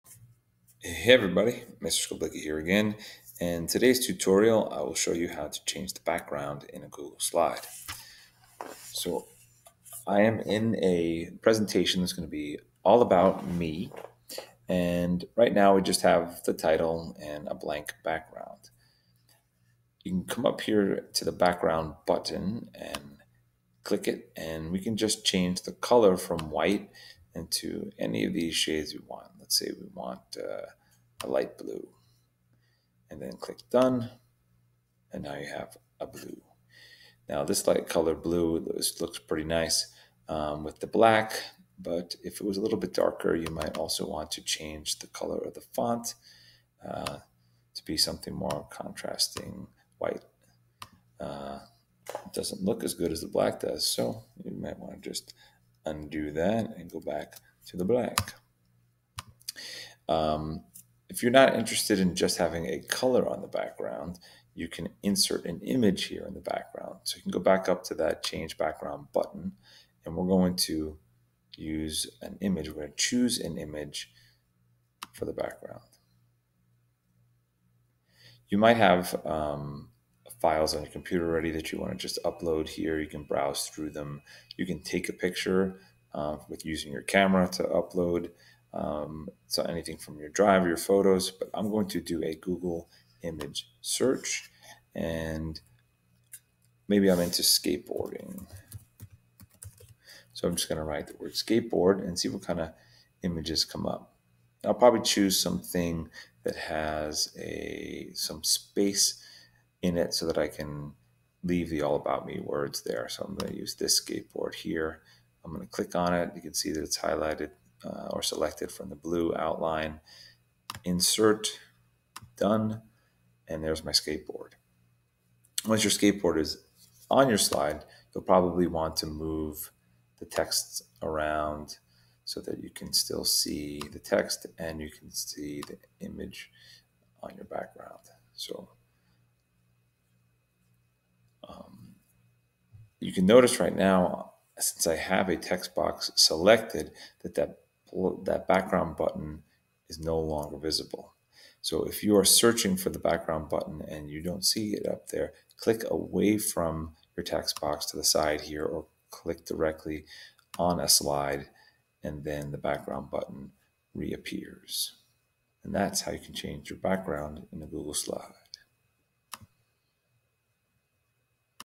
Google Slides - Change background (color or image)
This video teaches you how to change the background color or how to insert an image for a background in Google Slides.